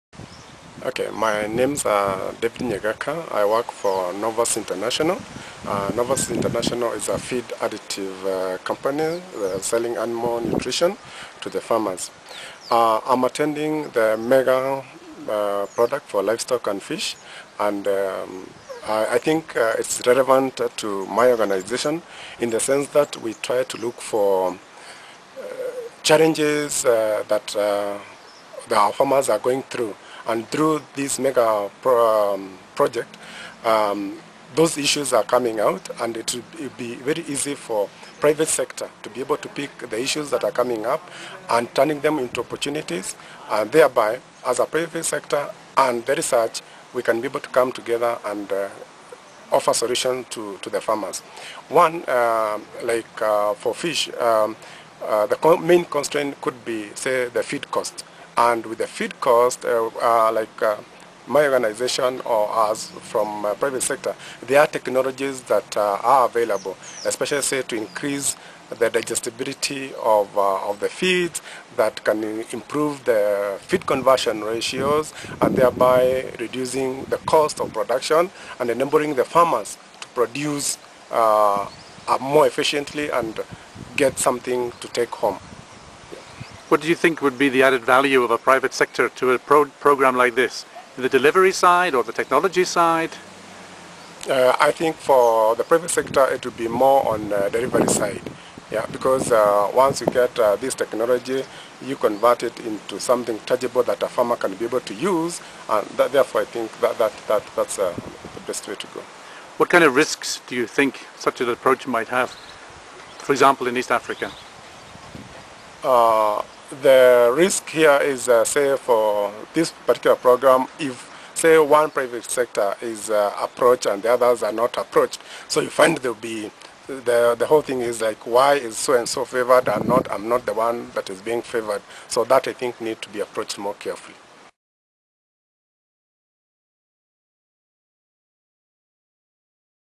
Private sector has an important role to play in Livestock Fish Mega Program
At the recent CGIAR Livestock Fish Mega Program consultation, David Nyagaka from Novus International argued that the private sector has an important role to play: Once it gets hold of technologies, it can convert them into something tangible which the farmers would be able to use. However, care is needed in approaching the private sector. He was speaking in the margins of the CGIAR Livestock Fish Mega Program stakeholder consultation meeting in Addis Ababa, 24 and 25 August 2010.